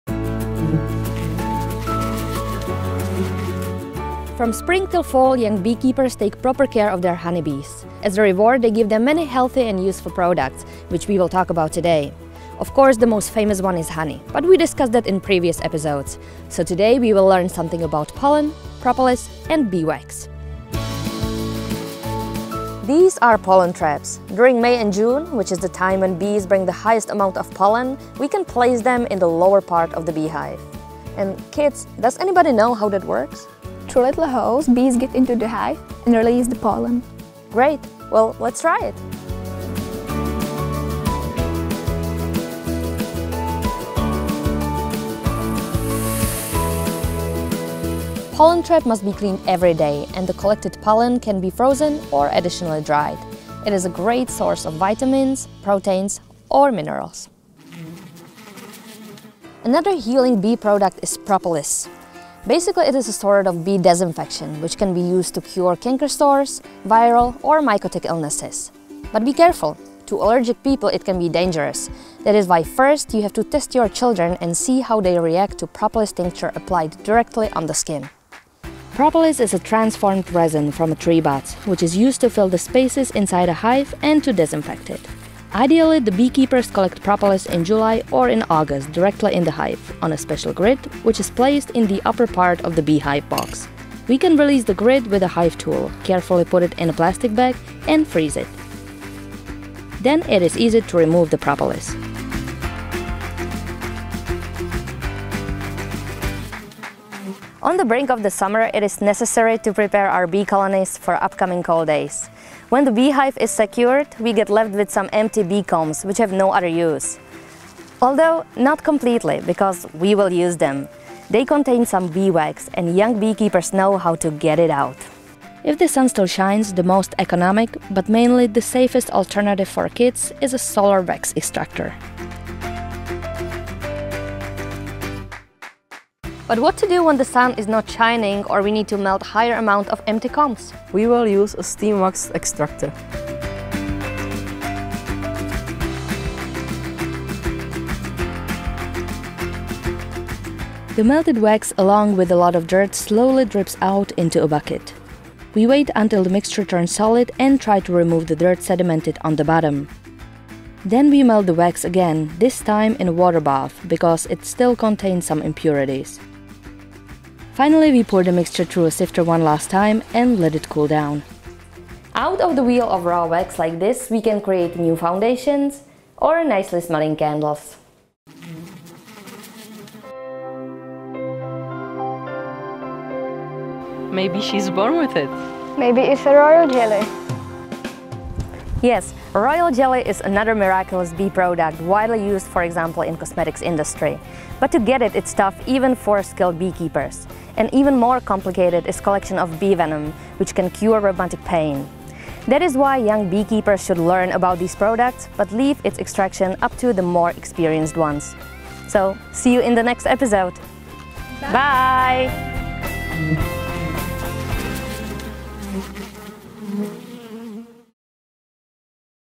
Bee Products
Honey is not the only important bee product. In this episode, we will show you how to collect other useful bee products – the pollen, wax, and propolis.